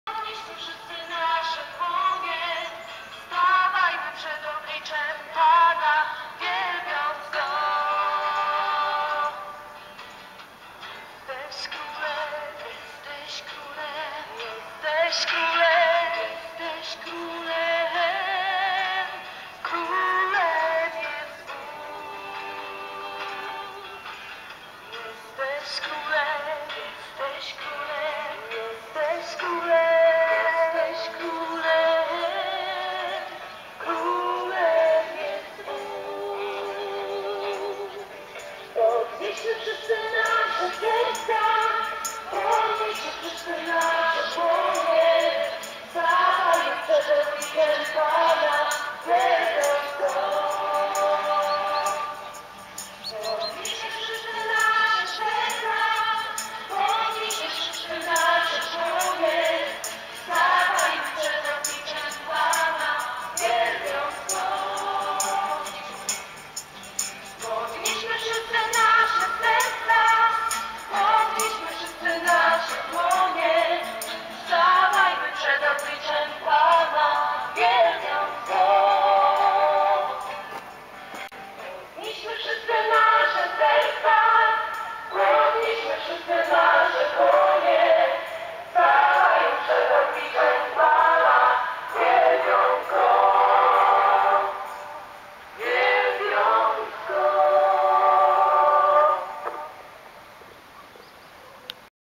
Pielgrzymka do Batisówki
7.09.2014 r. o 6 obudziły mnie żeńskie śpiewy. Początkowo myślałem, że to znad pobliskiego zalewu, ale po kilku minutach okazało się, e w kierunku Nadrzecza podąża bliżej nie znana pielgrzymka...